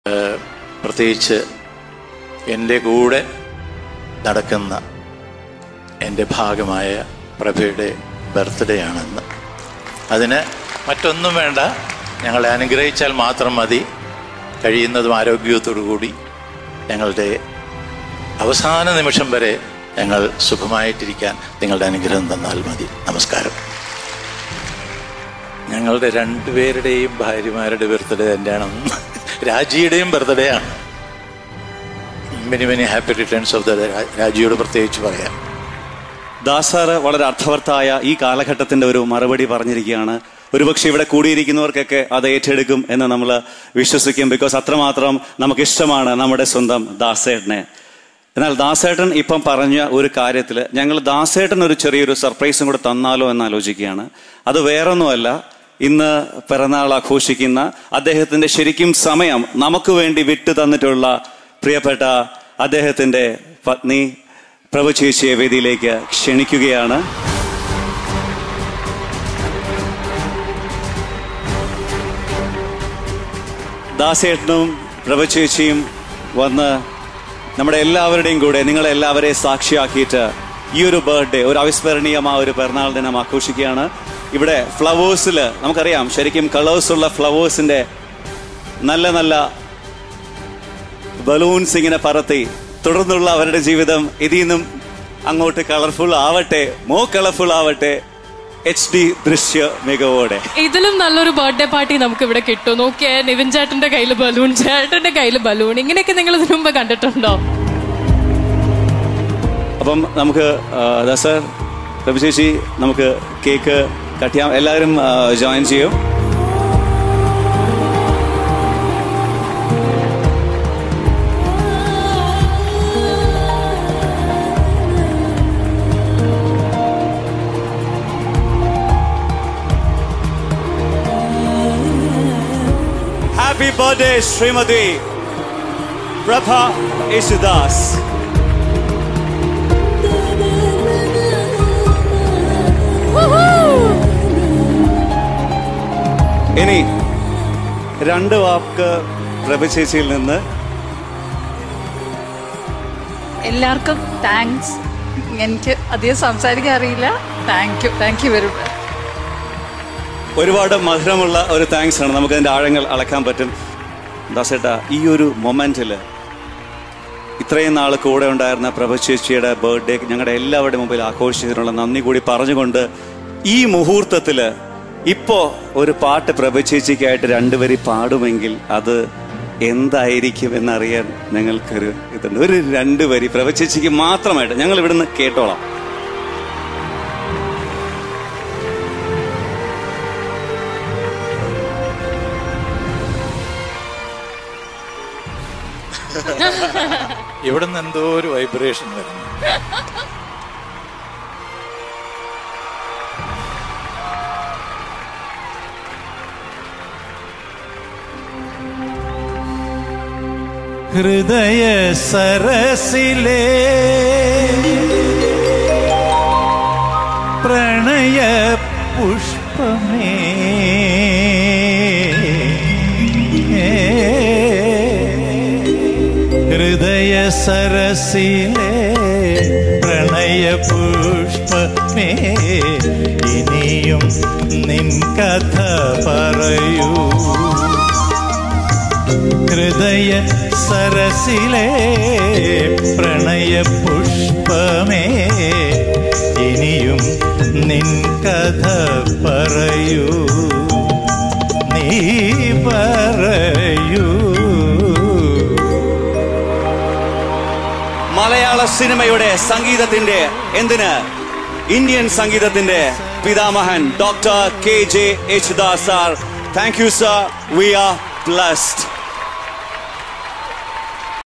KJ Yesudas celebrating his wife Prabha's Birthday at Flowers TV Music Award Function
Dasettan celebrating Prabha chechi's birthday .